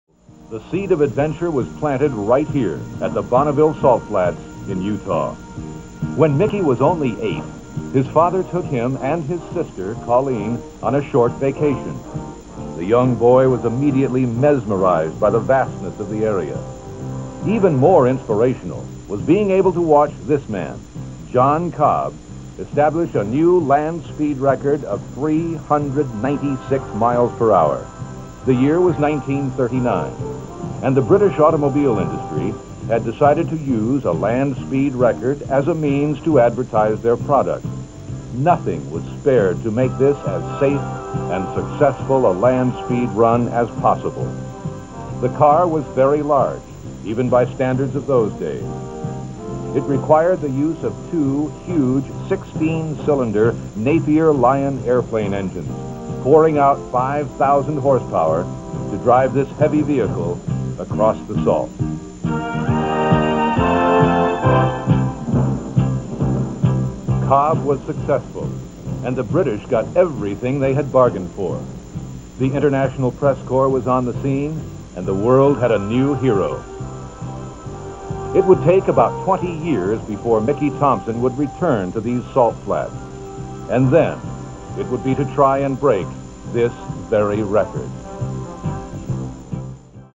Mickey Thompson A Vow is Made
When Mickey Thompson was eight years old, his father took him to the Bonneville Salt Flats in Utah to watch Englishman John Cobbs attempt to break the land speed record.

The British Automobile Industry wanted to use the new land speed record to promote their products. They had invested heavily in the design and building of the car which was large even for those days. It held two huge 16 cylinder Napier Lion aeroplane engines to drive the heavy vehicle across the salt.

Cobbs started his engine and thundered across the wide flat surface. Mickey Thompson looked on in awe as the huge car set a new record of 394.2 miles per hour. The international press was on the scene and the world had a new hero. As Mickey stood there on the salt flats beside his father, he vowed that one day he would beat that record.

Many years later, using all his savings, his determination and his courage, Mickey beat John Cobbs' record in his fifth attempt by travelling across the salt at 406.6 miles per hour in a car powered by four Pontiac engines and held together with spare parts from junkyards.

Breaking the land speed record was a high point in Mickey's racing career, and is perhaps his most important legacy.